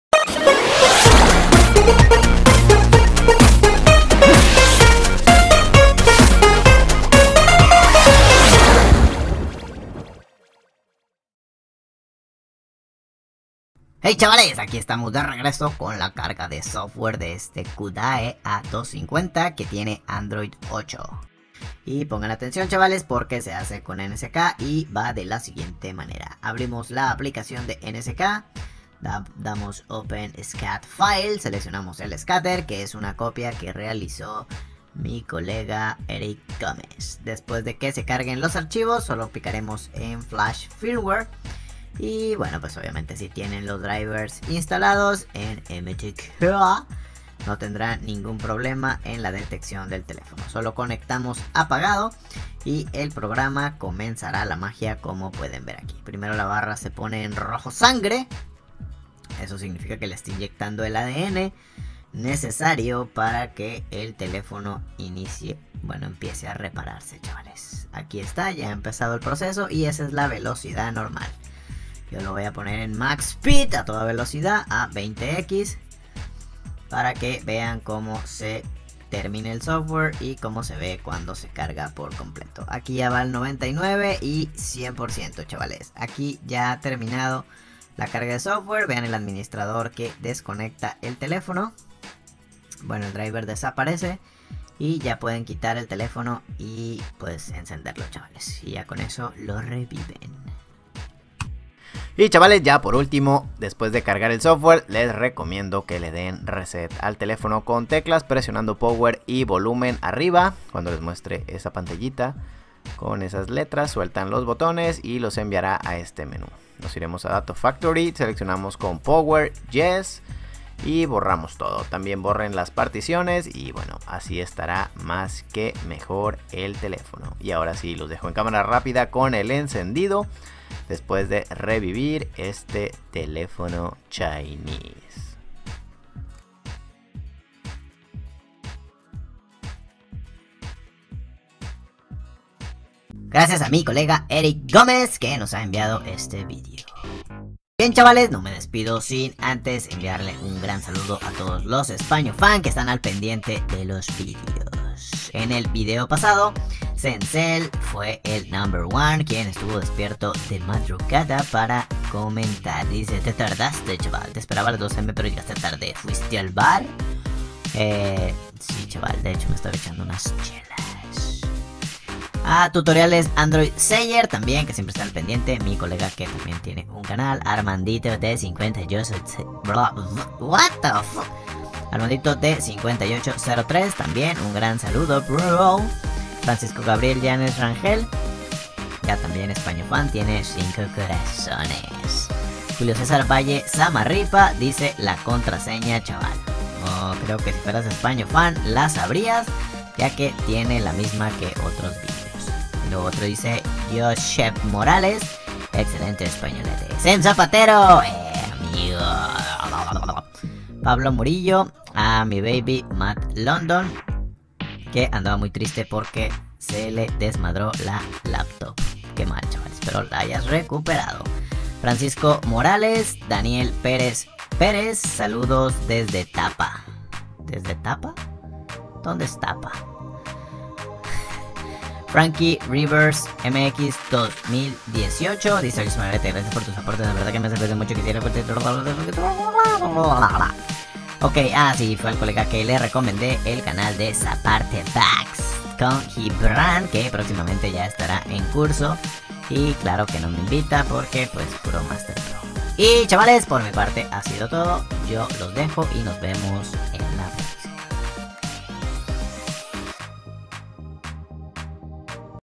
SOFTWARE KUDAE A250 - CON NCK
Hey Chavales les dejo este Firmware  para que lo puedan revivir sin problema con su NCK.

Links
SOFTWARE

http